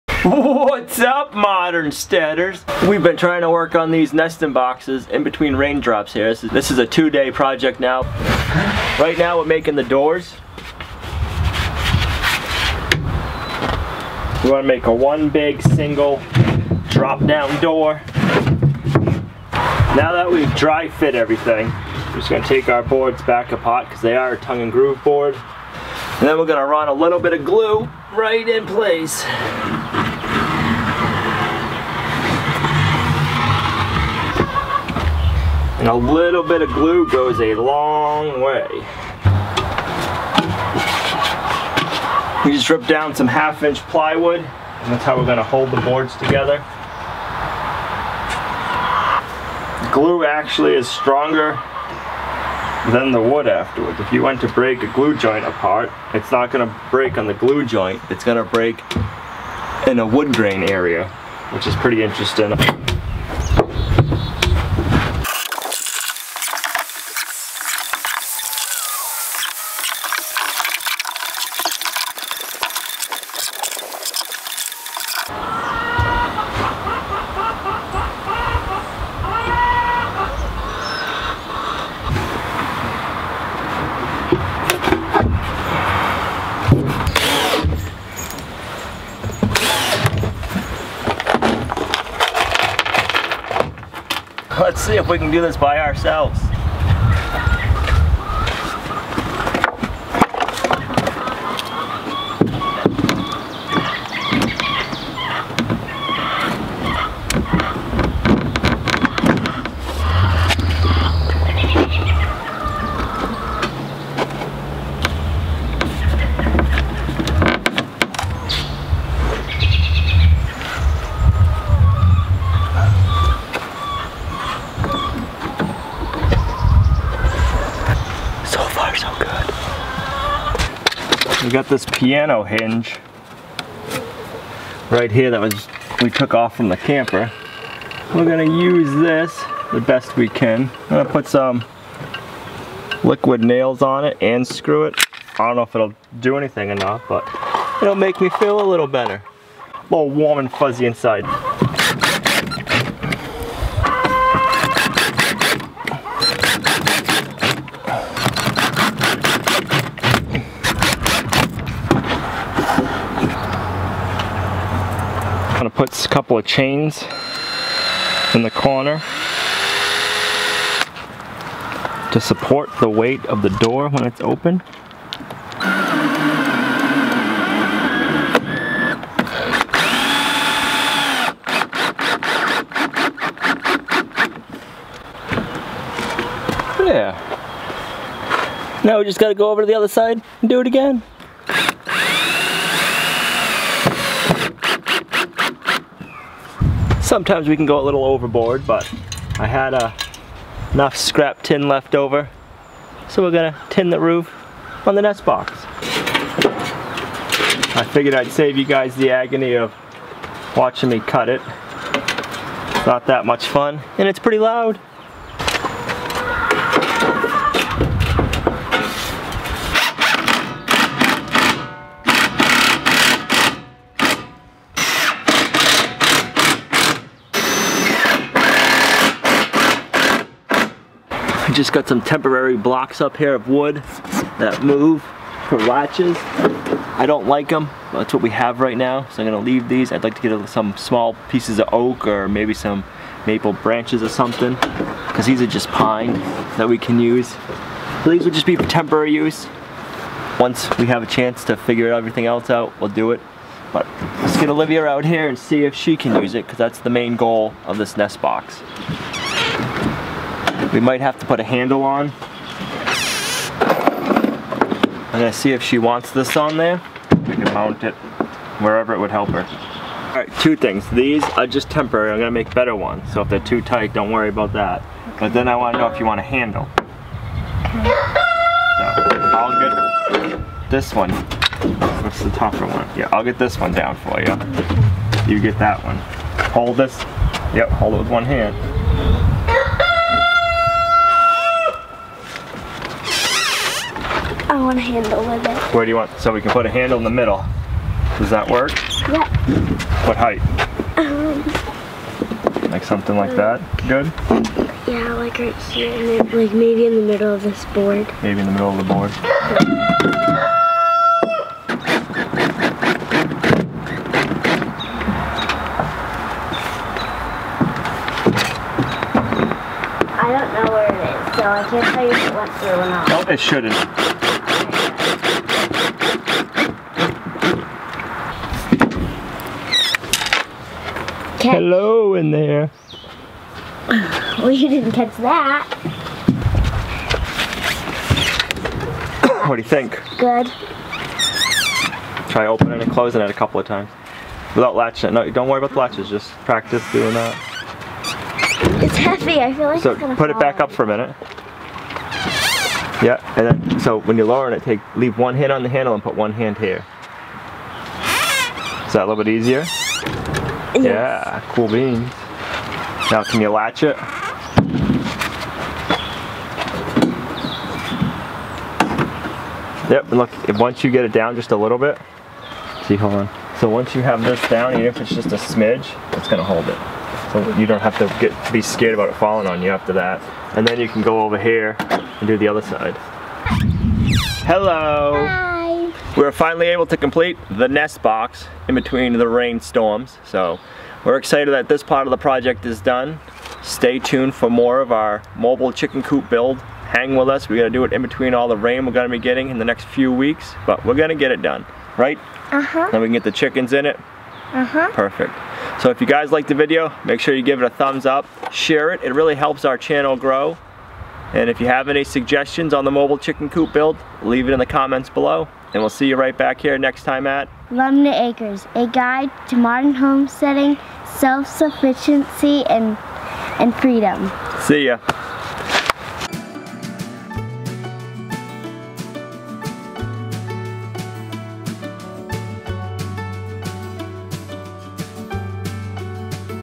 Finishing up the nest boxes Mobile Chicken Coop #6
The nest boxes are ready for use now we need to finish up the rest of the mobile chicken coop and get the laying hens in it! Pretty soon we will be collecting eggs from the nesting boxes.

Here is a nice Irwin Clamp set Like the ones we have been using!